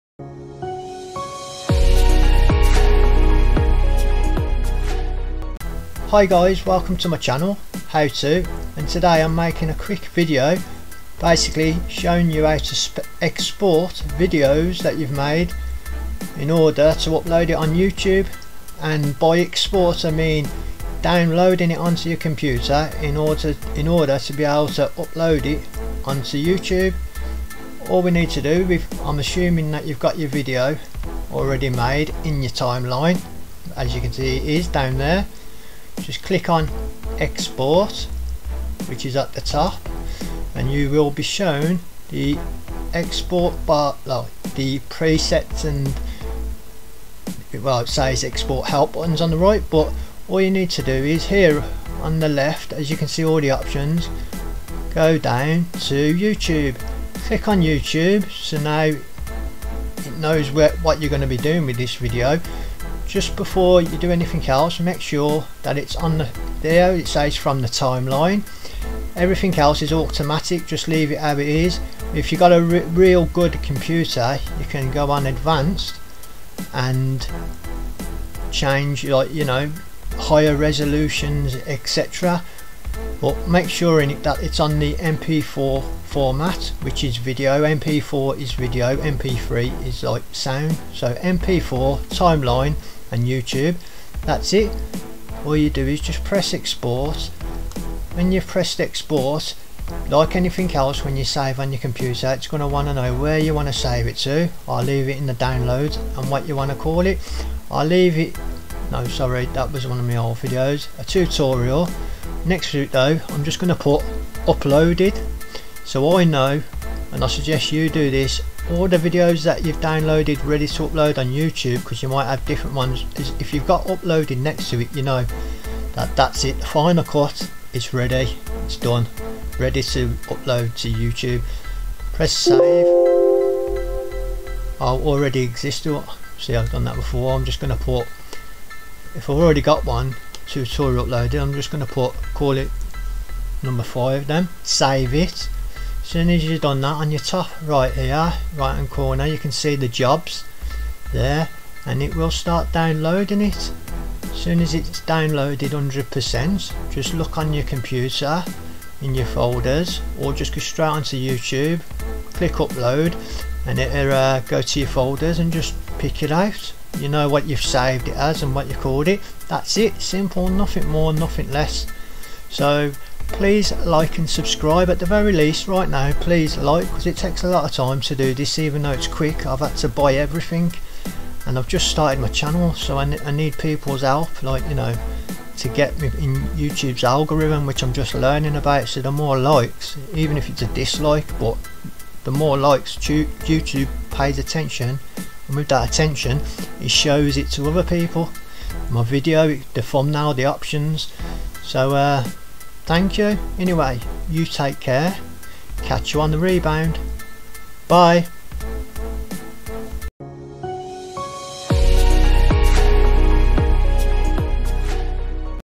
Shotcut Export For YouTube
How To Export Or Download Video Onto Your Laptop, Pc, Computer Or Any Other Device In 2 Mins.